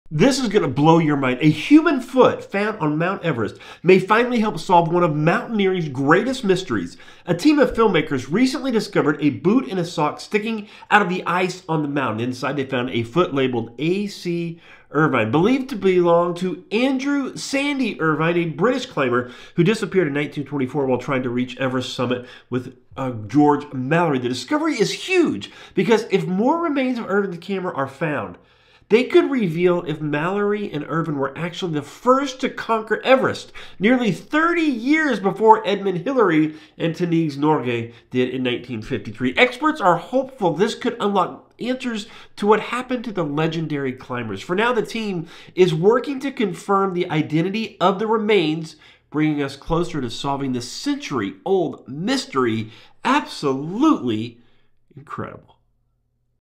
Foot found on Mount Everest may finally help solve one of mountaineering’s greatest mysteries
A human foot found on Mount Everest may finally help solve one of mountaineering’s greatest mysteries! A team of filmmakers recently discovered a boot and sock sticking out of the ice on the mountain. Inside, they found a foot labeled “A.C. Irvine,” believed to belong to Andrew “Sandy” Irvine, a British climber who disappeared in 1924 while trying to reach Everest’s summit with George Mallory.

The discovery is huge because if more remains or Irvine’s camera are found, they could reveal if Mallory and Irvine were actually the first to conquer Everest, nearly 30 years before Edmund Hillary and Tenzing Norgay did in 1953.

Experts are hopeful this could unlock answers to what happened to the legendary climbers. For now, the team is working to confirm the identity of the remains, bringing us closer to solving this century-old mystery!